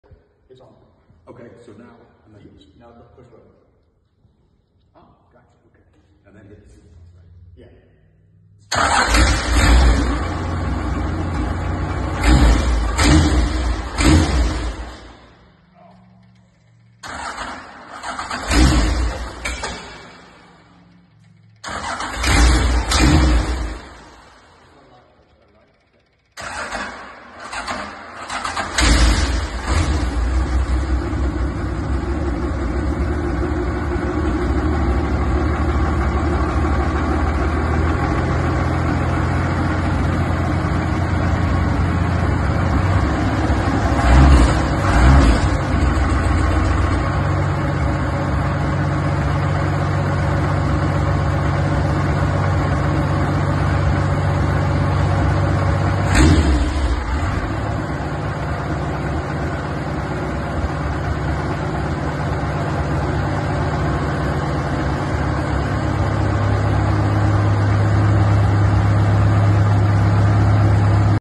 Tony Reed starting up the St. Jude tractor!!
Modified Truck and Tractor Pulling

For more content like this:

Check us out on Facebook - @Beer Money Pulling Team